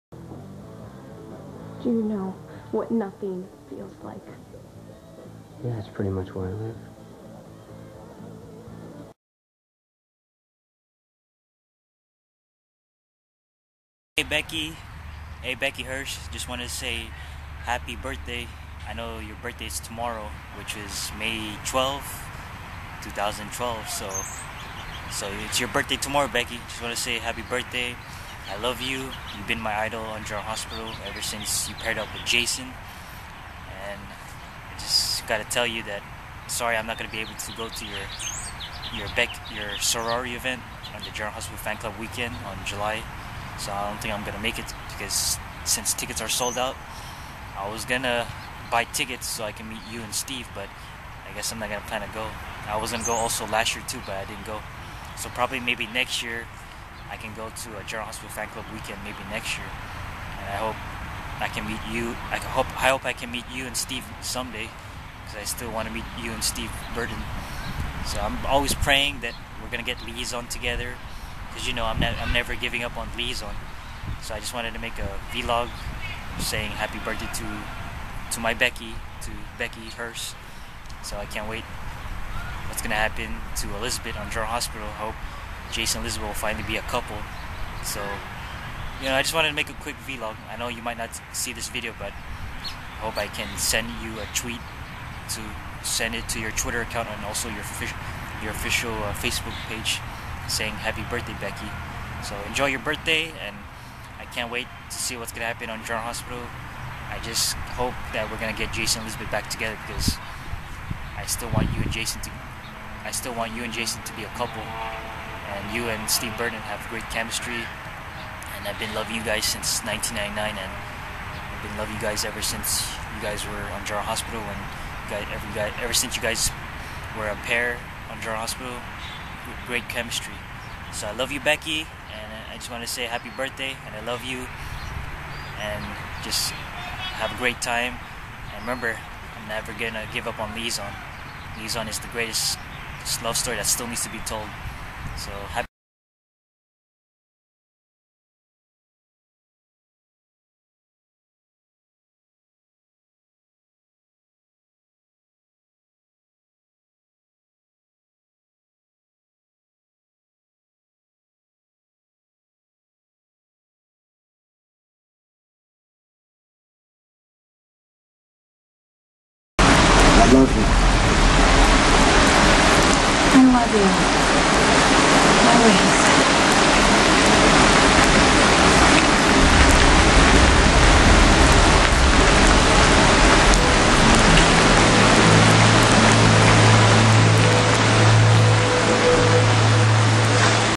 GH Elizabeth Becky Herbst Happy Birthday & Happy Mother's Day!
At 2:26 it stops so forward it and go 2:34 for the next slide. Sorry if its not that perfect my video because something was wrong with my camera! If you watch this video Becky sorry if you can only see half of my face because I was recording my birthday greeting Vlog to you at the park in the morning. Happy 35th Birthday Becky turning on May 12, 2012 and born in 1977! Have fun celebrating your birthday, have a great mother's day and still never giving up on Liason because they deserve to be a couple on 2012!
XOXO
Love
Bruce your fan who will always love you on GH! Without you on GH, Elizabeth is nothing without me worth watching!